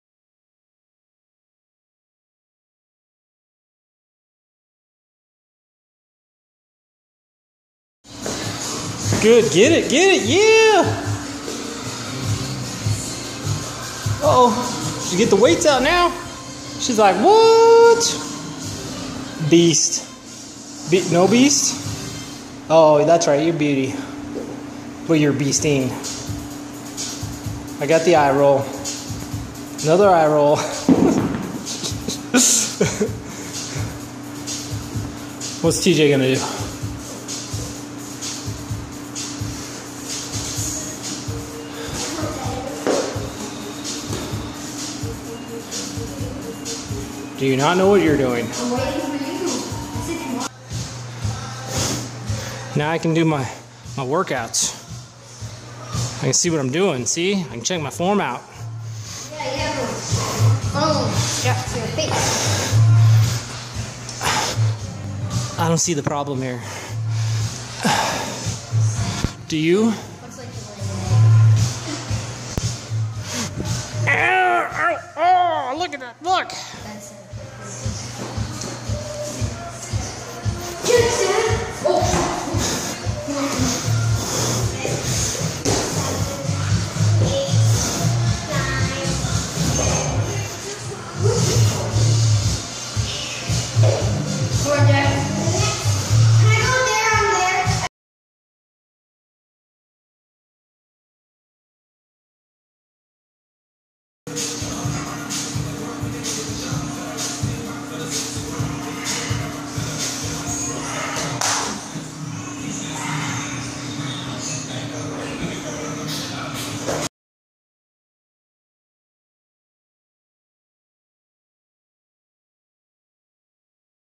The family the works out together, stays together! Boom!
The family the works out together, stays together! Momma doing her thing training a client while we got our workout on! Our girly can do 50 box jumps and 500 meters on the rowing machine (both her first time), and our boy did 1000 meters on the rowing machine. Both can do proper form push ups too! These two rock some work!